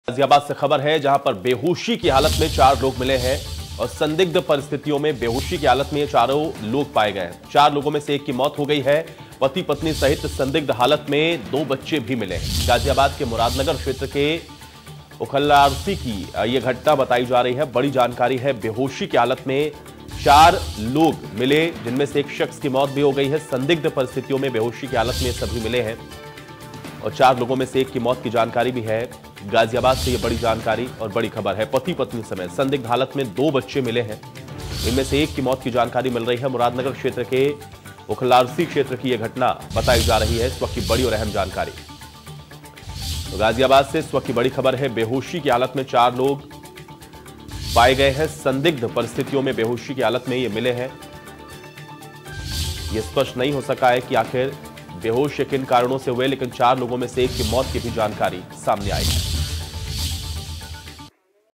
Ghaziabad : संदिग्ध परिस्थितियों में बेहोश मिले एक ही परिवार के 4 लोग, इलाके में मचा हड़कंप
Ghaziabad : संदिग्ध परिस्थितियों में बेहोश मिले एक ही परिवार के 4 लोग, इलाके में मचा हड़कंप | India News UP